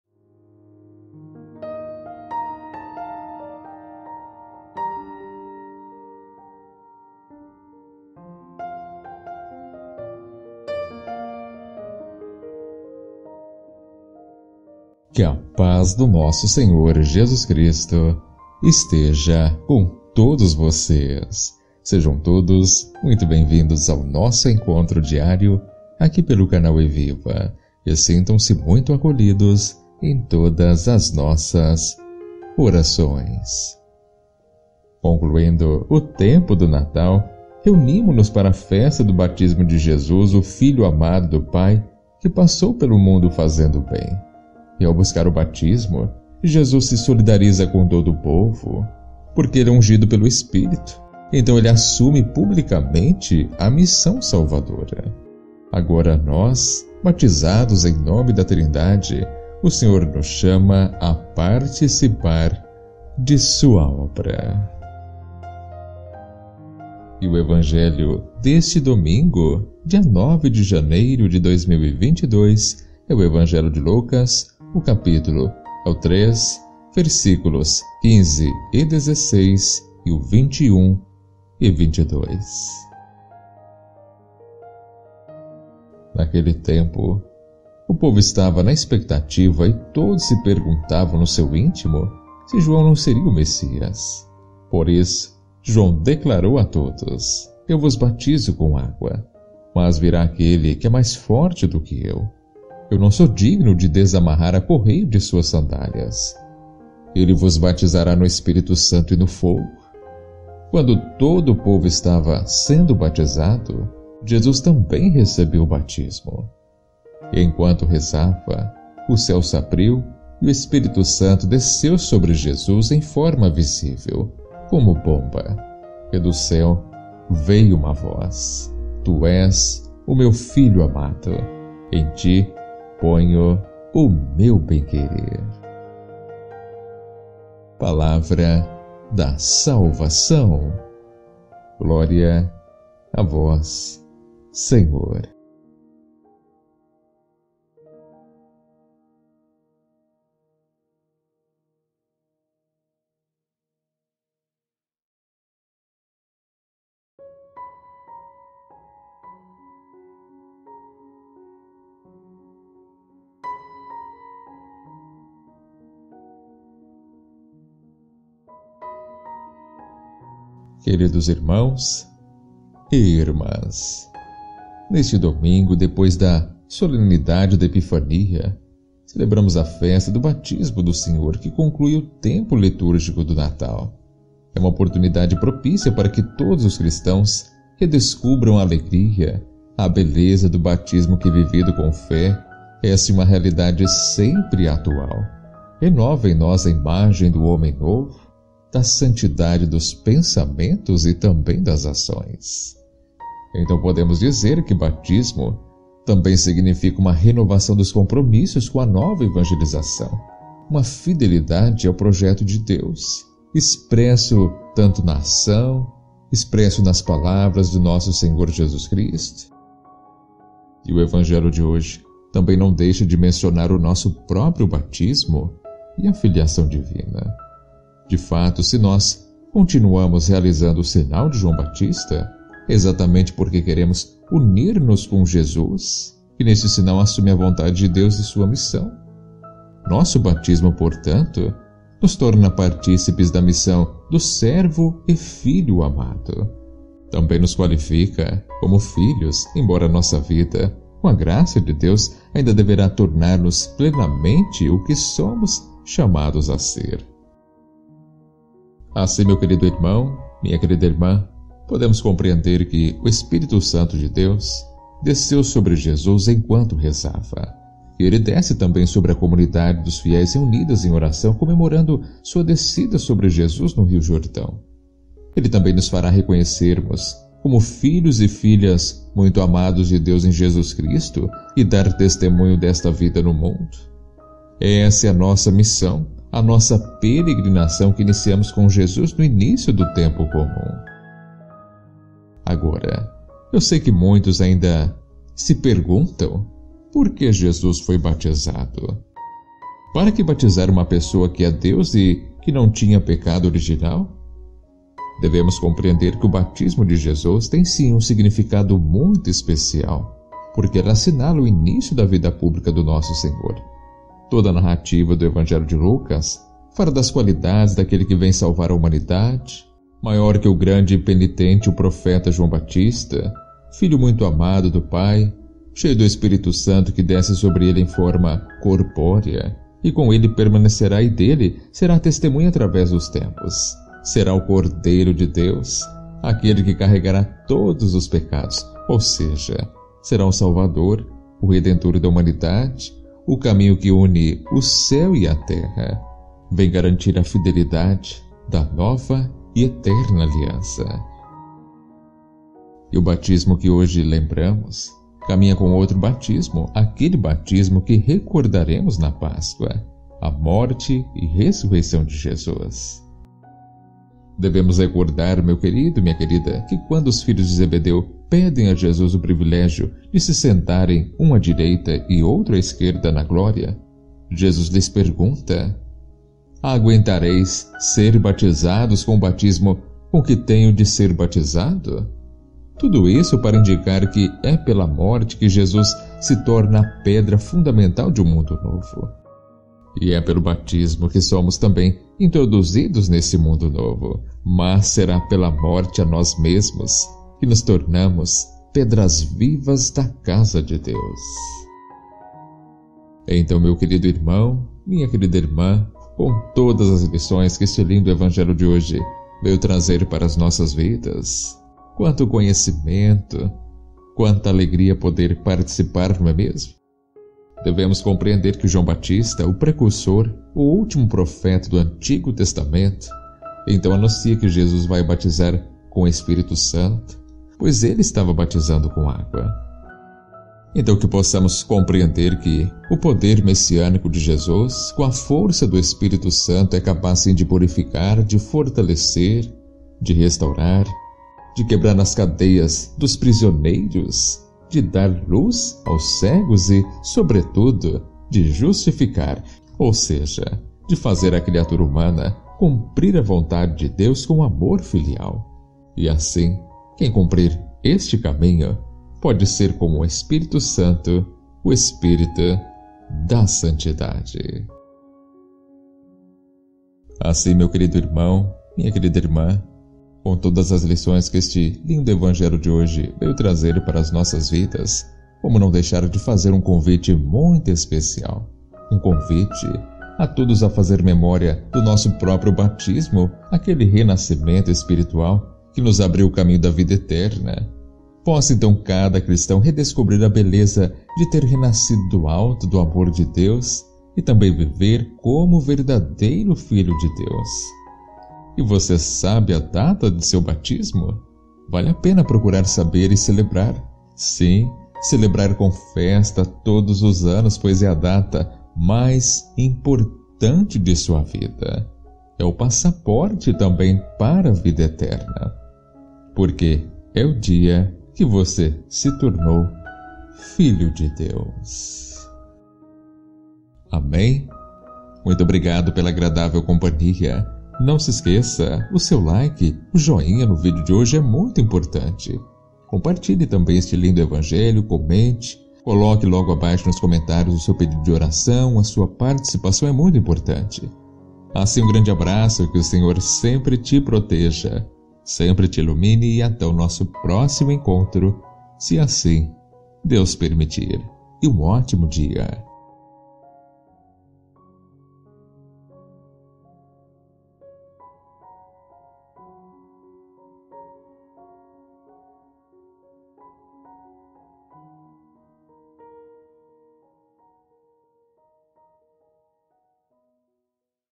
EVANGELHO DO DIA 09/01/2022 Narrado e Comentado LITURGIA DIÁRIA HOMILIA DIARIA HOJE ORAÇÃO eVIVA
Evangelho do dia de hoje DOMINGO DIA 9 DE JANEIRO DE 2022 Bom dia eVIVA COM REFLEXÃO DE VIDA | EVANGELHO DE NOVEMBRO É LUCAS 3,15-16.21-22. BATISMO DO SENHOR.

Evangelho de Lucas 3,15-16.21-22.
Naquele tempo, 15o povo estava na expectativa e todos se perguntavam no seu íntimo se João não seria o Messias. 16Por isso, João declarou a todos: “Eu vos batizo com água, mas virá aquele que é mais forte do que eu. Eu não sou digno de desamarrar a correia de suas sandálias. Ele vos batizará no Espírito Santo e no fogo”. 21Quando todo o povo estava sendo batizado, Jesus também recebeu o batismo. E, enquanto rezava, o céu se abriu 22e o Espírito Santo desceu sobre Jesus em forma visível, como pomba. E do céu veio uma voz: “Tu és o meu Filho amado, em ti ponho o meu benquerer”. – Palavra da salvação.